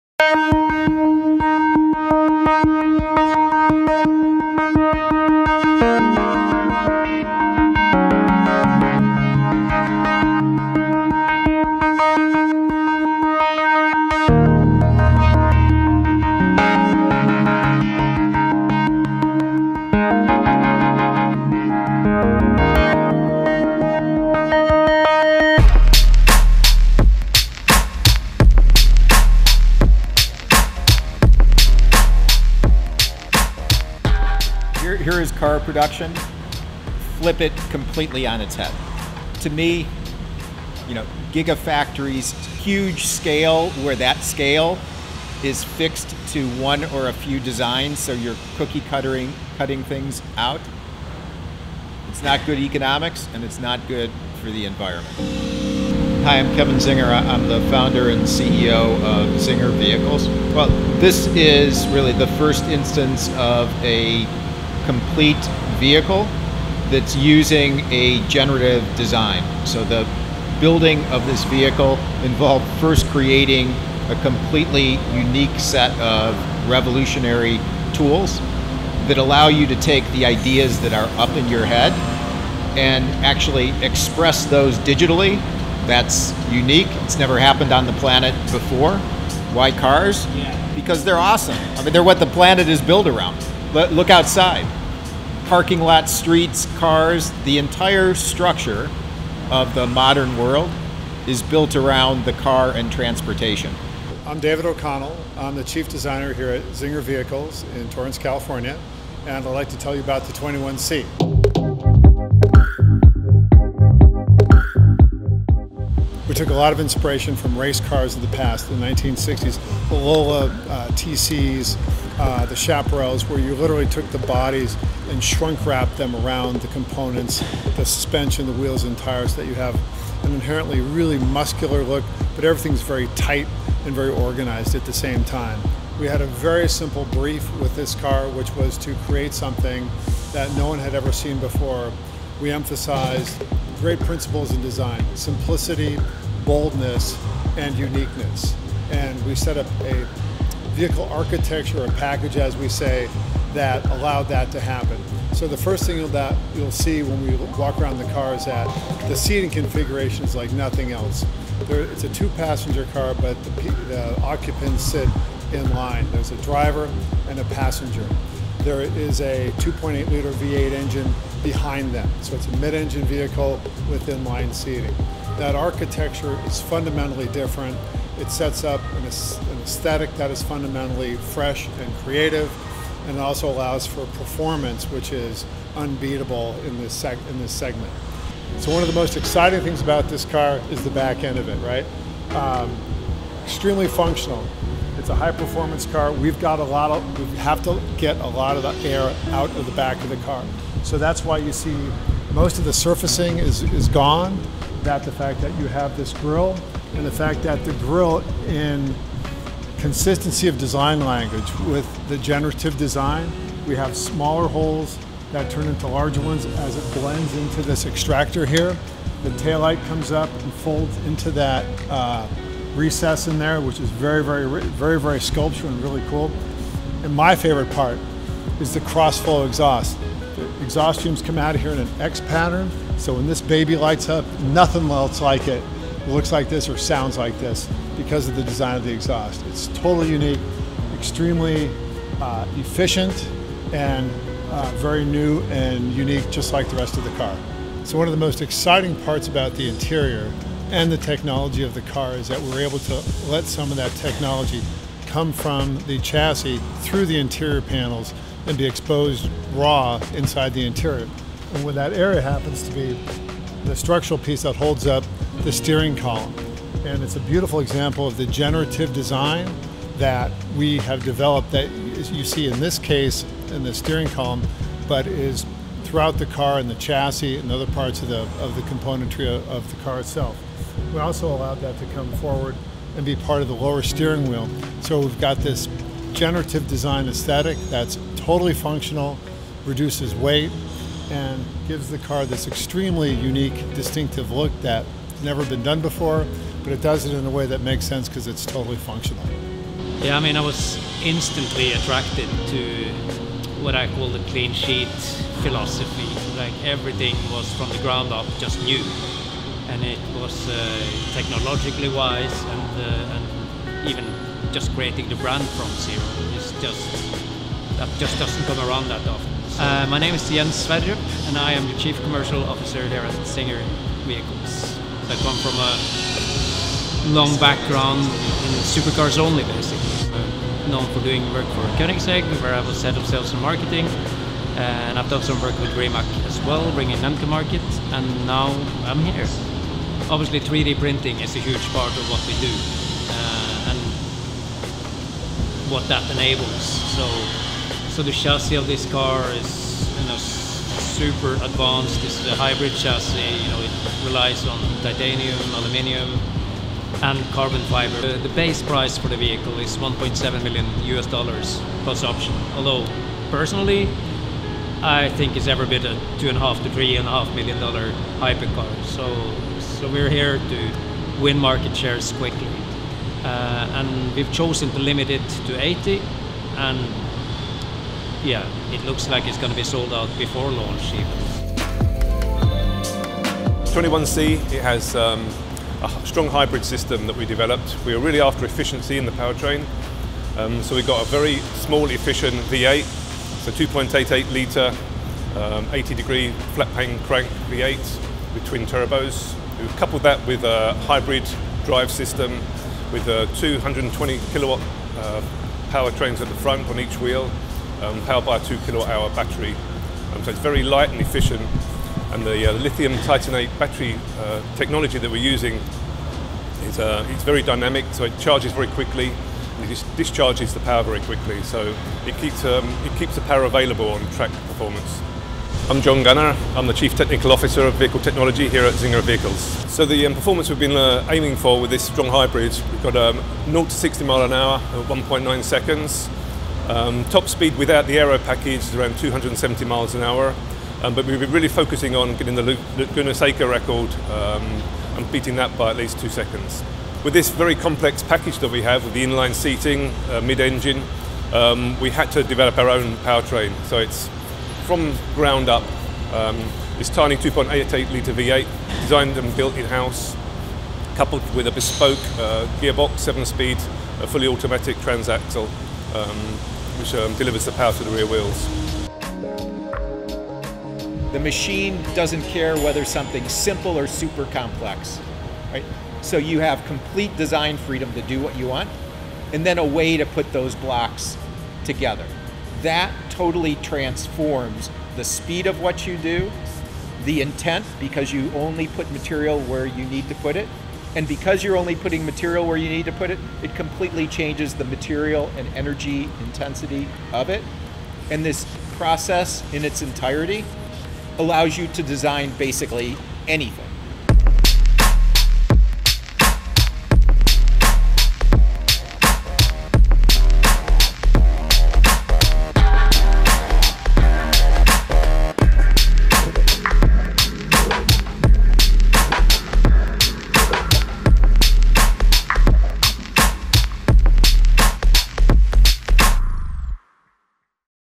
Czinger 21C: The 270mph 3D-printed Hypercar
We flew to LA to get an exclusive preview of perhaps one of the most interesting cars of the last five years. This 270mph machine is revolutionary in the way that it is digitally designed and robotically assembled. A true leap forward in automotive innovation and a glimpse into the future.